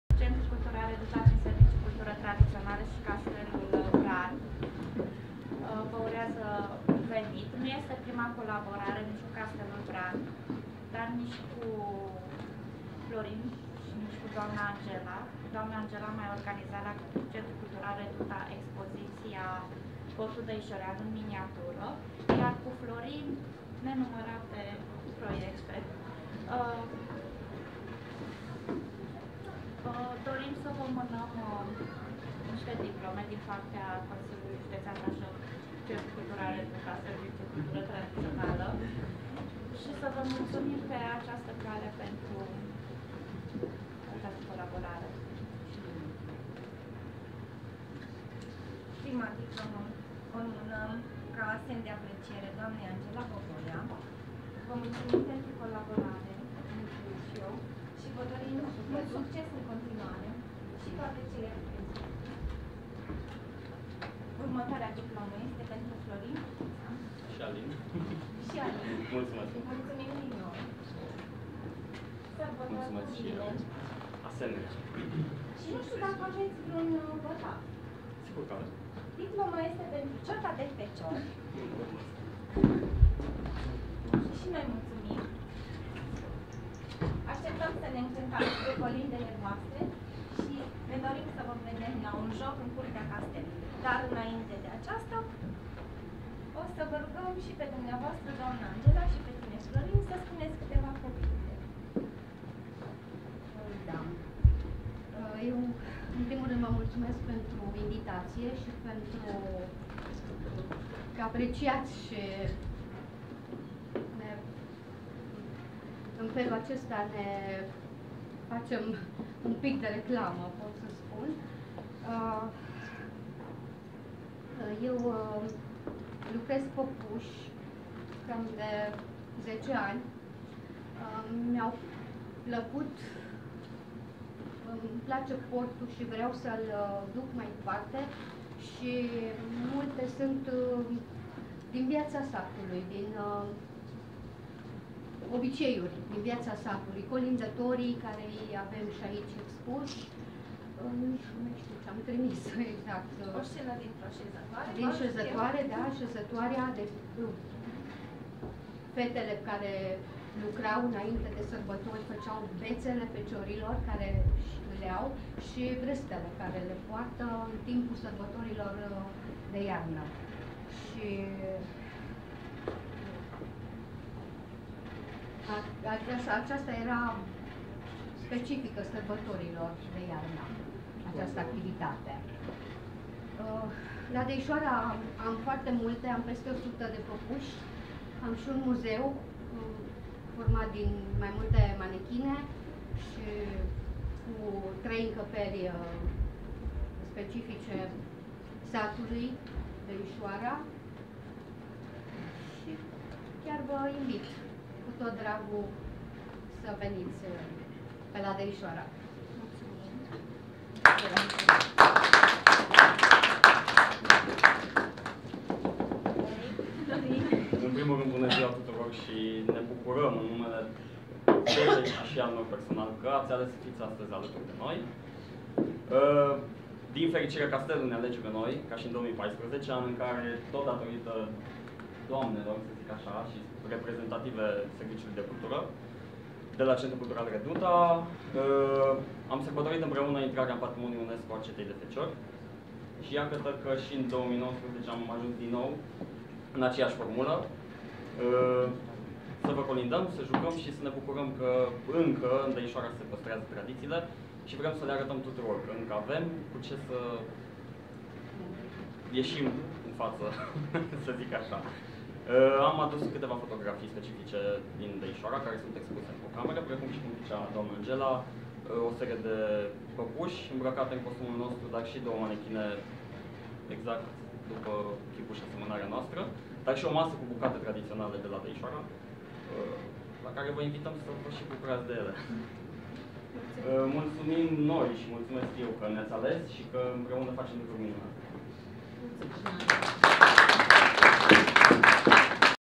Vernisaj expoziţia ,,Colindăm, Doamne, Colind!"
Expoziţia deschisă vizitatorilor Castelului Bran până în data de 13 ianuarie 2020, cuprinde: un interior ţărănesc din zona etnografică Rupea, colecţia de păpuşi - parte integrantă a proiectului ,,Portul dăişorean în miniatură" iniţiat şi dezvoltat de Angela Boboia, precum şi o expoziţie de fotografii realizate de Florin Bucuţea - "Dăişoara - locuri, oameni şi sărbători".

Ceata de feciori de la Dăişoara a susţinut un program artistic de colinde şi jocuri specifice zonei. Înregistrare 21 decembrie 2019.